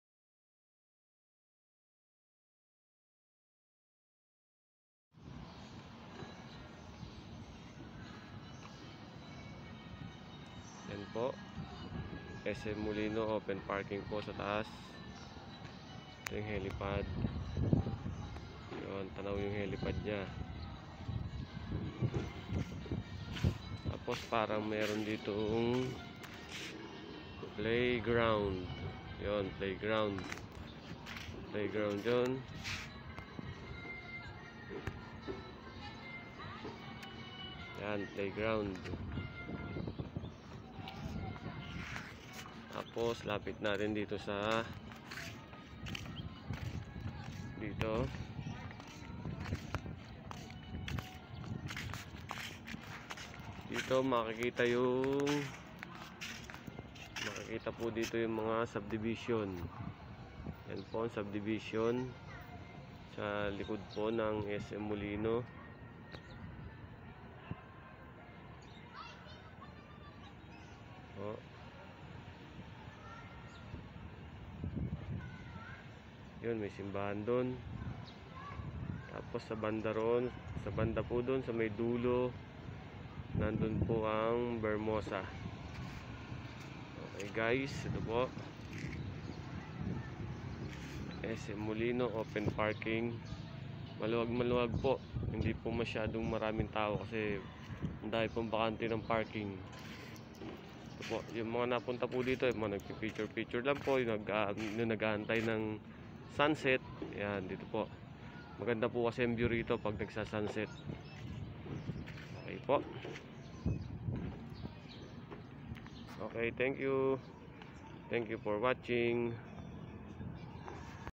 Anong Hiwaga Meron sa Open parking ng Sm molino bakit ito pinipuntahan ng mga tao
Ok alright dito tayo ngayon sa open parking ng sm city molino sa bacoor..ano ba meron dito